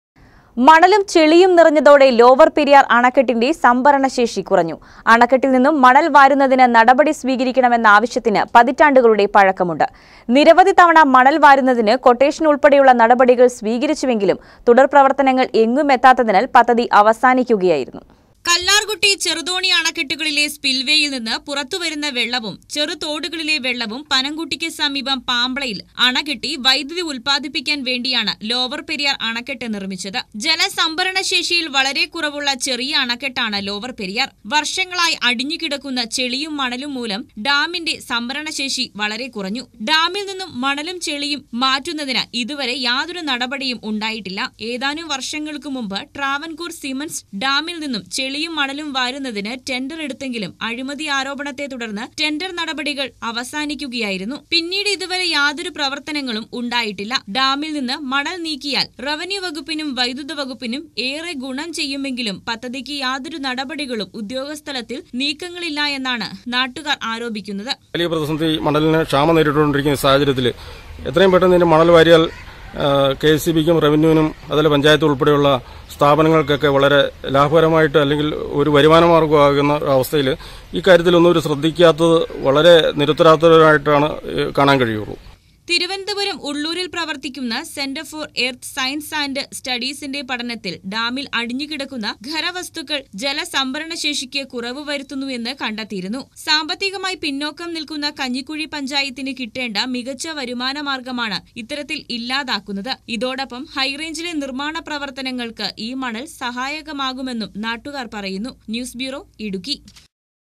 ലോവര്‍പെരിയാര്‍ അണക്കെട്ടിന്റെ സംഭരണശേഷി കുറഞ്ഞു.
മണലും ചെളിയും  നിറഞ്ഞതോടെ ലോവര്‍പെരിയാര്‍ അണക്കെട്ടിന്റെ സംഭരണശേഷി കുറഞ്ഞു. അണക്കെട്ടില്‍ നിന്നും മണല്‍ വാരുന്നതിന് നടപടി സ്വീക  രിക്കണമെന്ന ആവശ്യത്തിന് പതിറ്റാണ്ടുകളുടെ പഴക്കമുണ്ട്. നിരവധി തവണ മണല്‍ വാരുന്നതിന് ക്വൊട്ടേഷന്‍ ഉള്‍പ്പെടെയുള്ള നടപടികള്‍ സ്വീകരിച്ചു എങ്കിലും തുടര്‍പ്രവര്‍ത്തനങ്ങള്‍ എങ്ങുമെത്താത്തതിനാല്‍ പദ്ധതി അവസാനിക്കുകയായിരുന്നു.